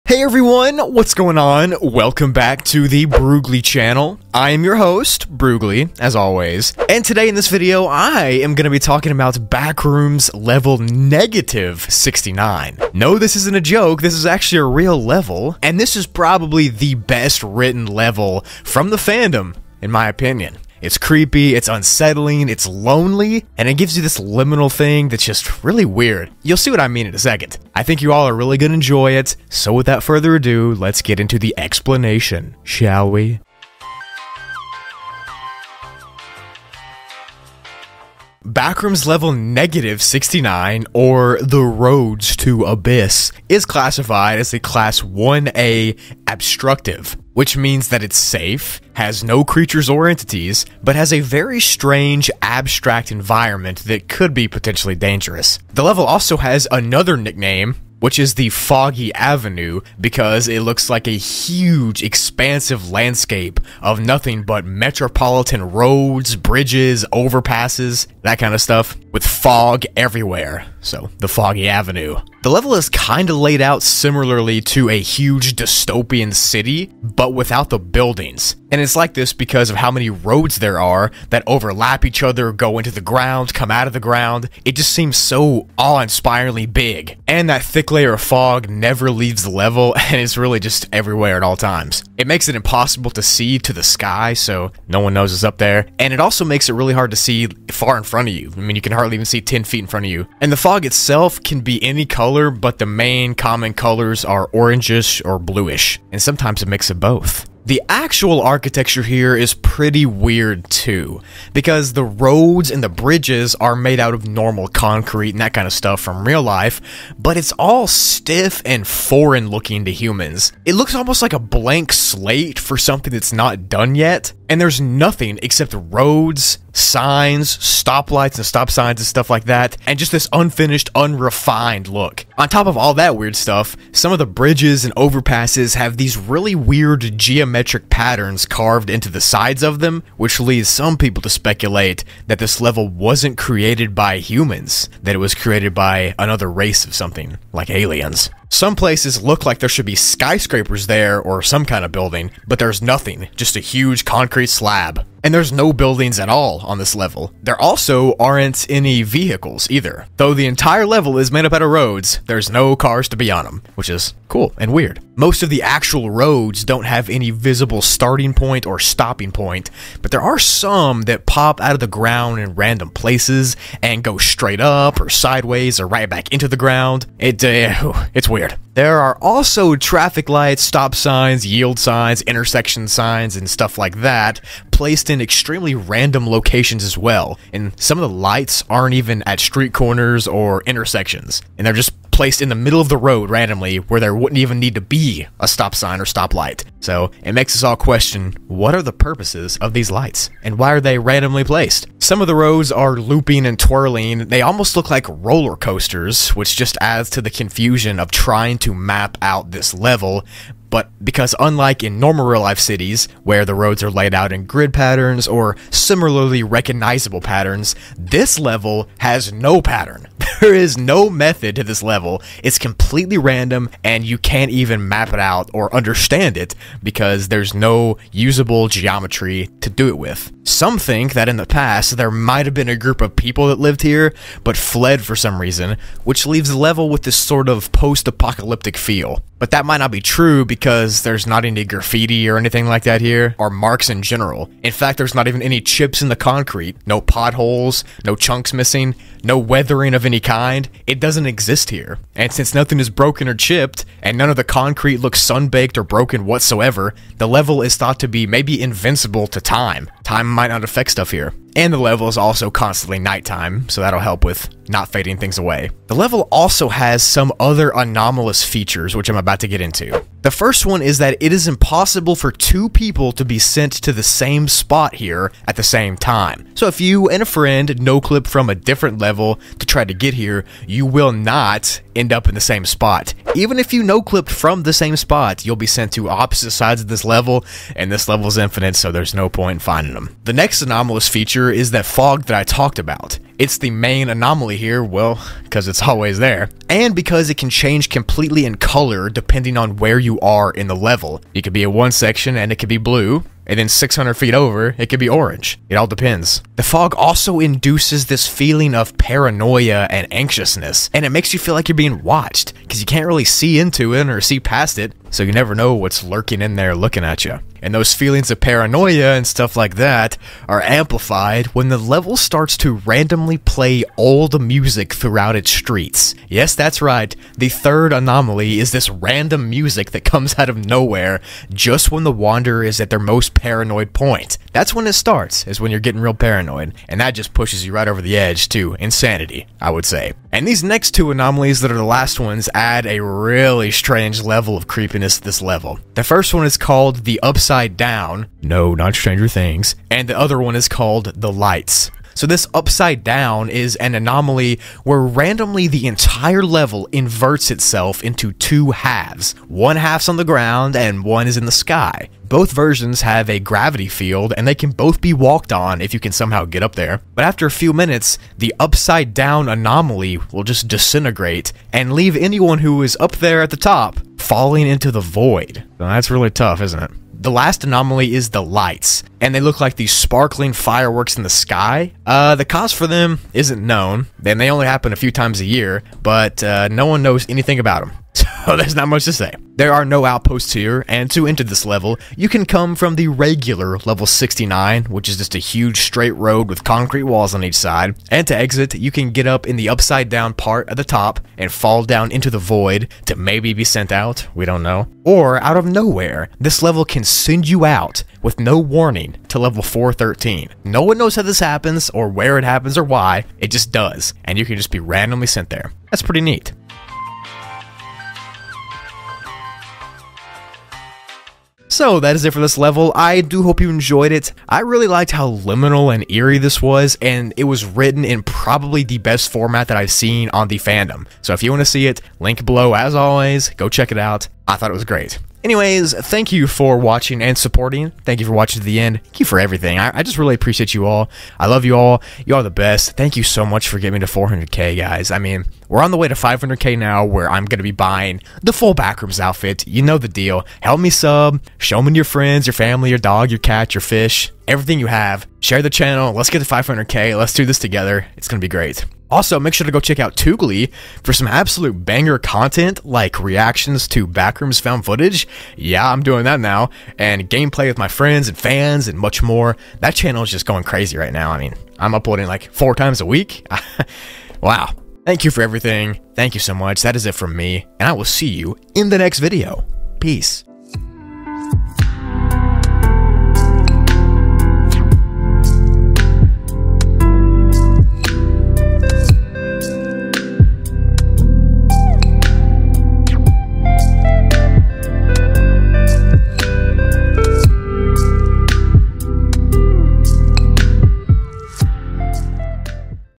The DANGERS of Backrooms level -69
Hey all! In this video I talk about a very mysterious and cryptic Backrooms level called "The Roads To Abyss" or Backrooms level -69... this one is the best written level on the fandom (in my opinion) and I think you all will like its creepiness

Intro 0:00
Explanation 0:33
Outro 7:00